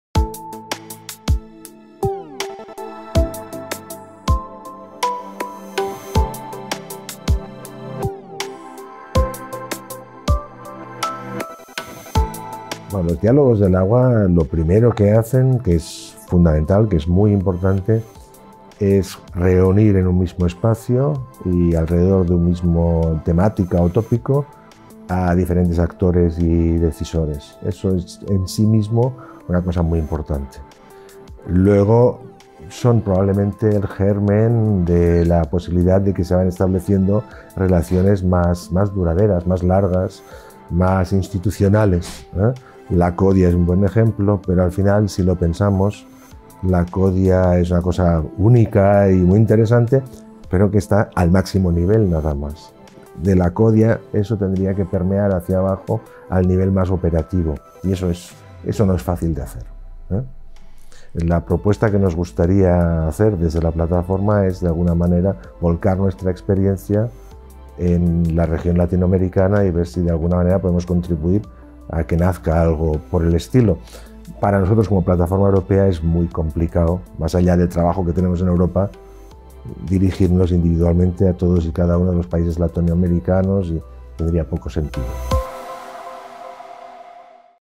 Tomás Michel: "Los Diálogos del Agua reúnen en un mismo espacio a diferentes actores y decisores"
La Casa de América de Madrid acogió el pasado 23 de octubre la IV edición de los Diálogos del Agua América Latina-España, un evento de discusión temática que se realiza cada año y que sirve de punto de encuentro entre América Latina y España para promover el intercambio de experiencias y conocimientos hídricos.

El evento forma parte de una plataforma estratégica de colaboración de largo plazo conformada por CAF - banco de desarrollo de América Latina y el Reino de España, a través de los Ministerios para la Transición Ecológica (MITECO), Economía y Empresa (MINECO) y Asuntos Exteriores, Unión Europea y Cooperación (MAEUEC), y crea un entorno de debate sobre el ciclo urbano del agua, la economía circular y la innovación en tecnologías hídricas, entre otros temas.

En el evento, iAgua habló con Tomás Michel, presidente la Plataforma Tecnológica del Agua Potable y Saneamiento, UE.